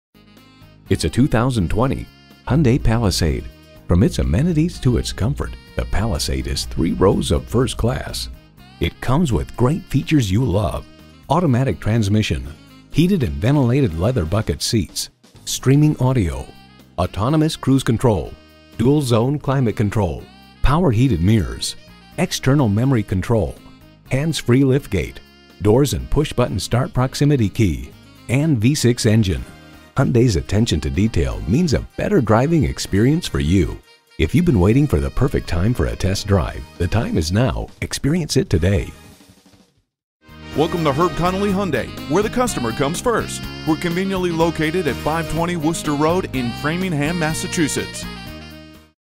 New 2020 Hyundai Palisade Framingham, MA #17055 - SOLD
Year: 2020
Make: Hyundai
Model: Palisade
Trim: Limited
Engine: 3.8L 6-Cylinder
Transmission: 8-Speed Automatic with SHIFTRONIC
Color: Gray
Interior: Black
Stock #: 17055
VIN: KM8R5DHE2LU034126

Address:
520 Worcester Rd - Rte 9 East